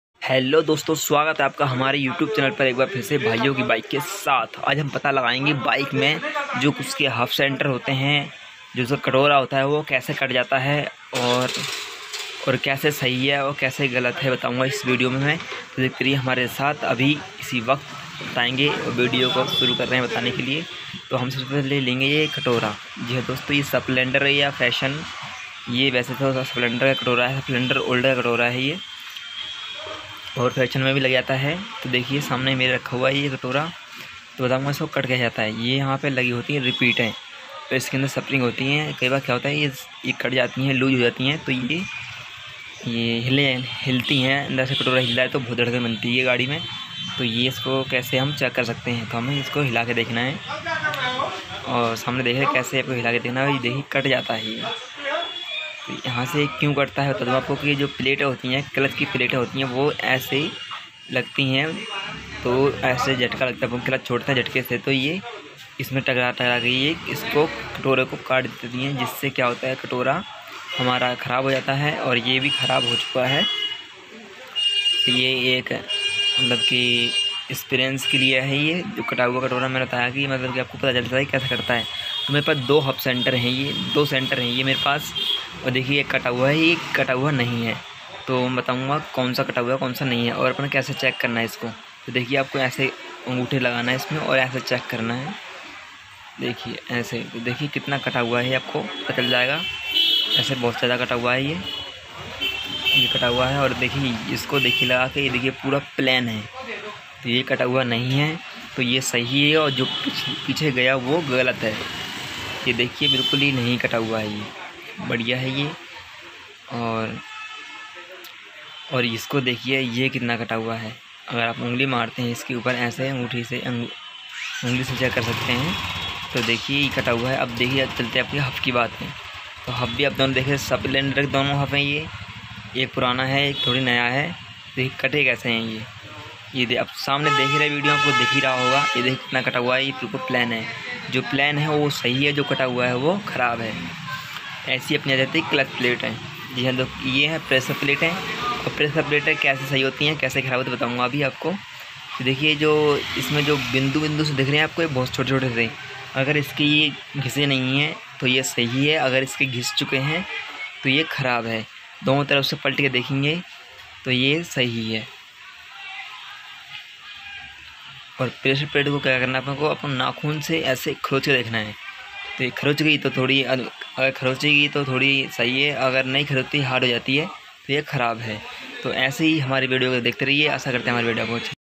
bike ke halfcentre kese check kar sakte hai अपने हाथों से खुद चेक कर सकते हैं
hello 200 swagat aapka Mari to bhaiyon bike ke sath aaunga aapko ki aap centre ko kaise check kar sakte hain lekin hamare Sath hamare YouTube channel par aayega